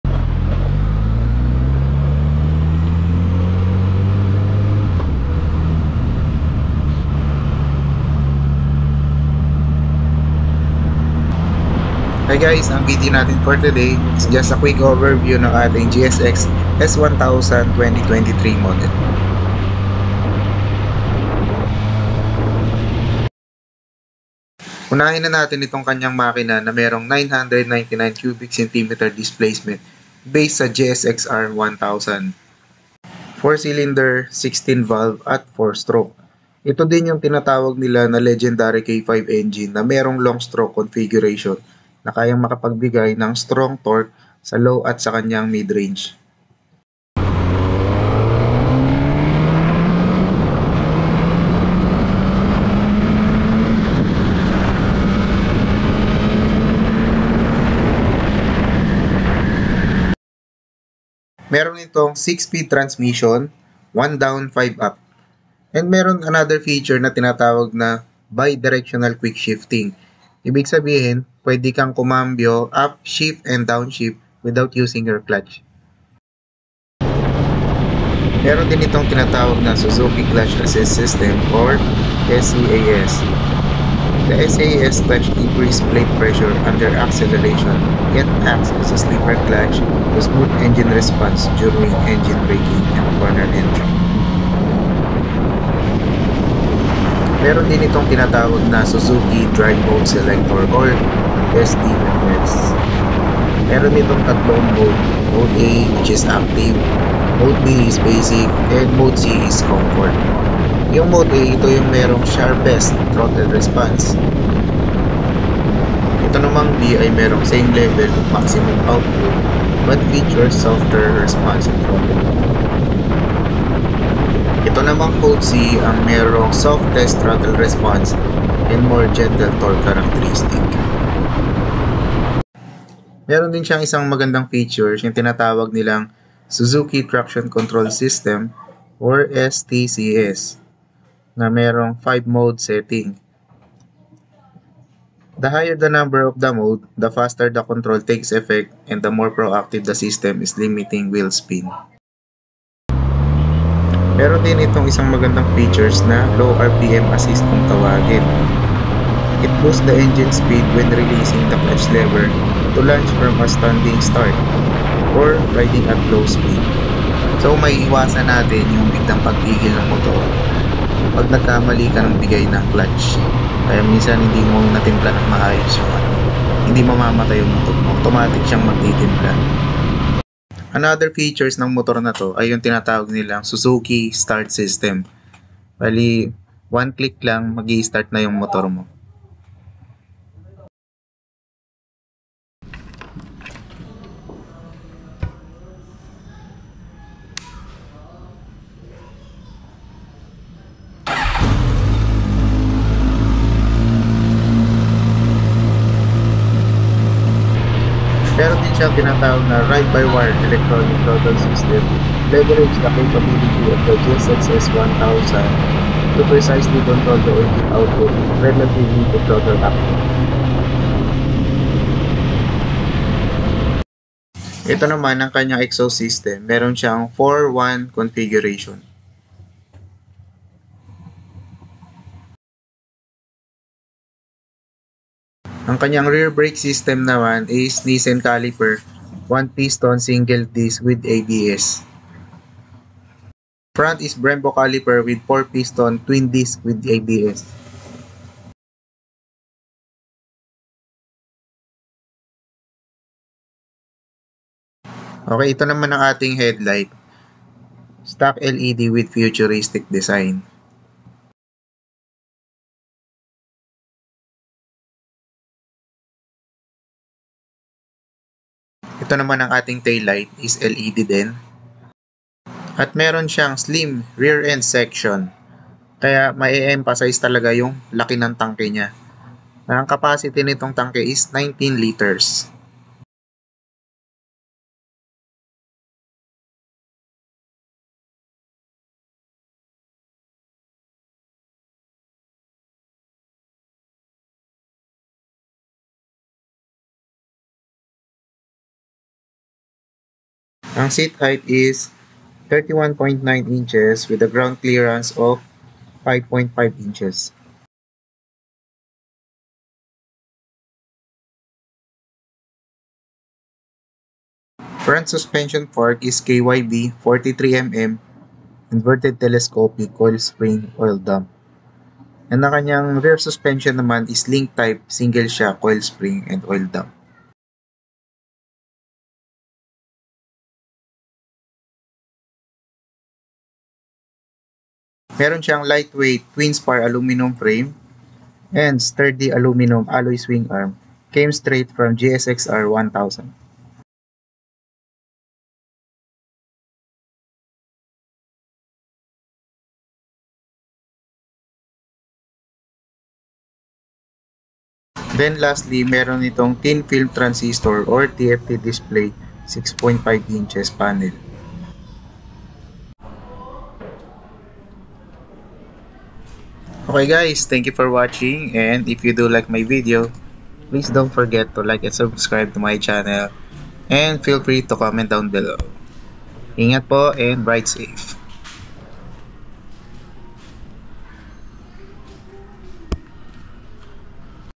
GSX-S1000 / 2023 / QUICK OVERVIEW / PJ_86
Just a quick overview on GSX-S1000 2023 model. SOme feature and specs